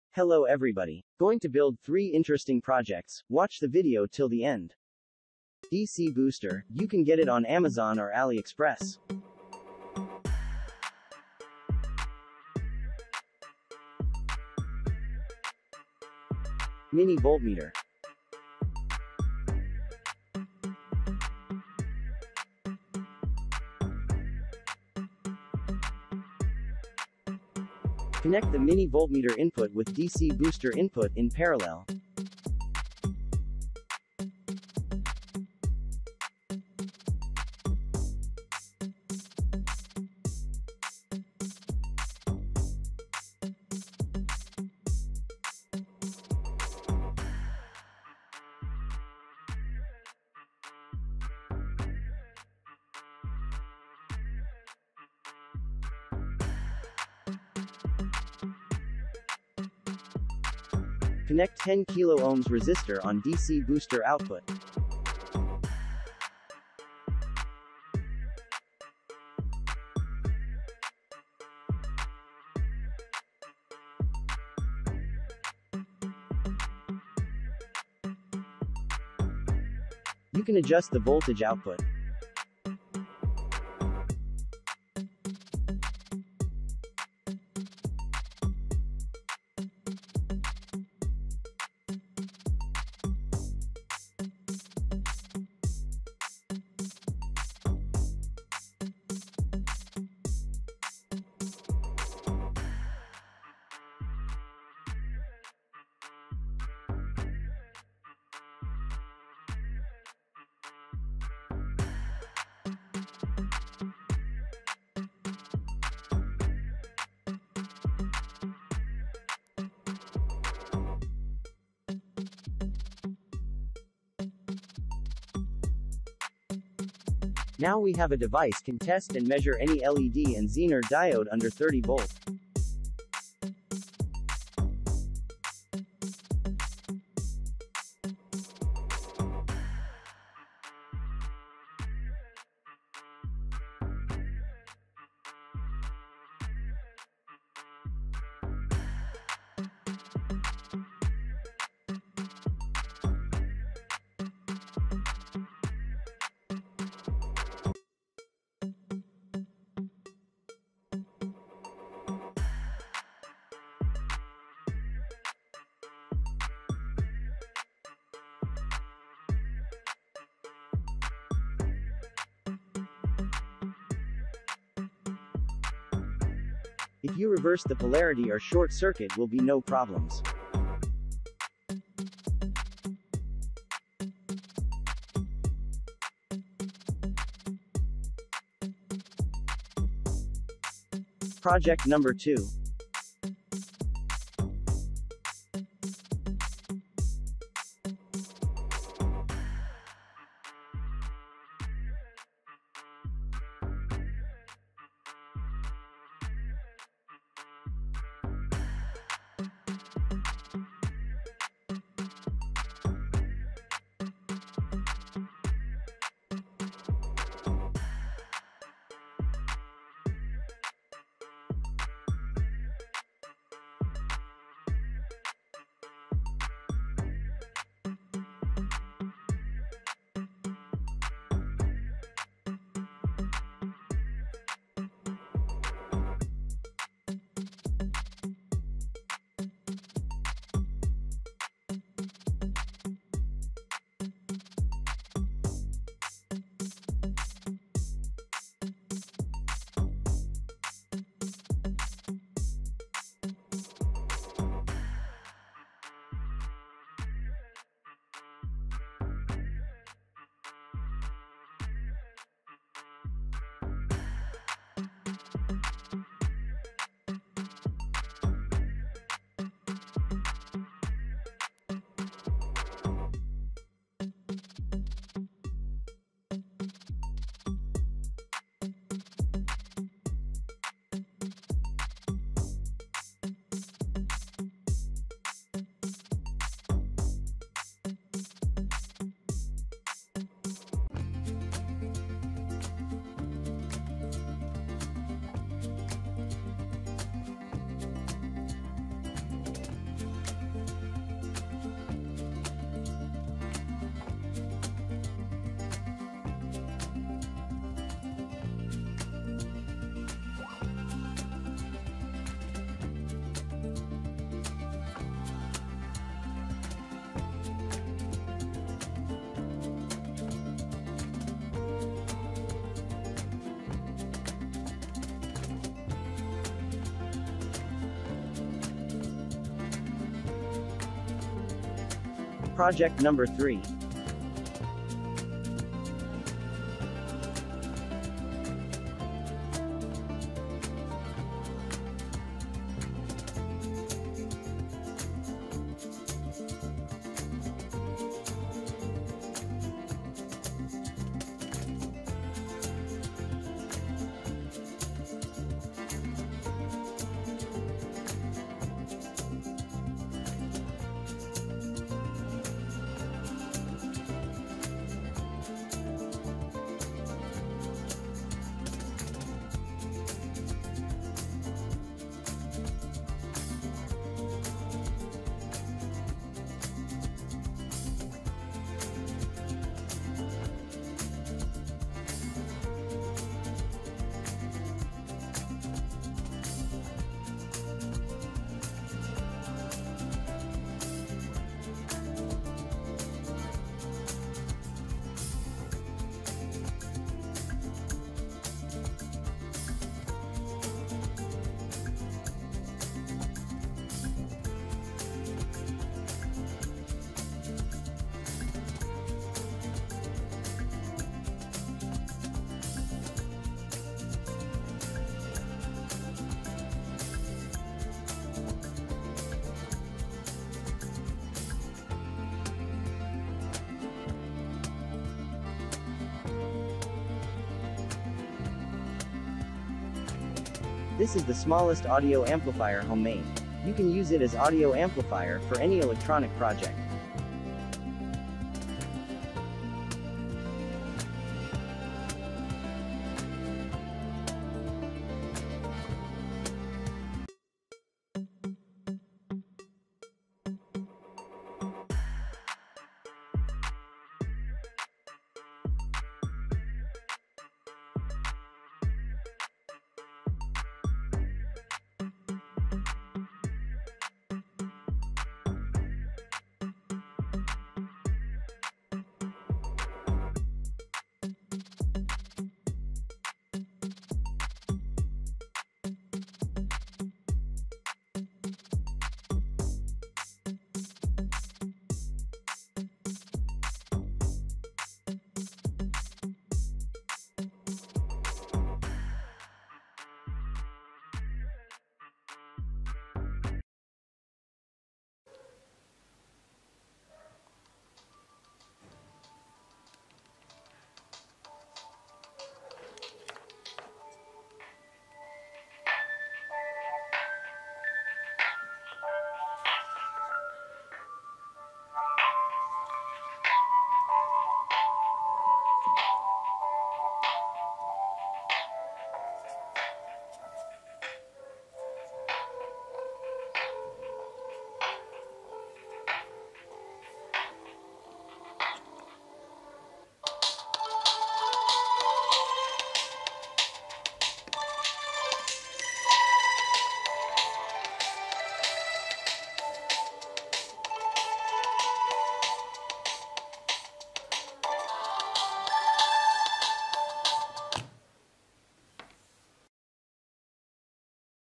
Amazing Projects ,, You Can Make At Home
3 Electronics projects :
- any LED & zener diode Tester
-NPN & PNP Tester using Timer 555
-the Smallest Audio Amplifier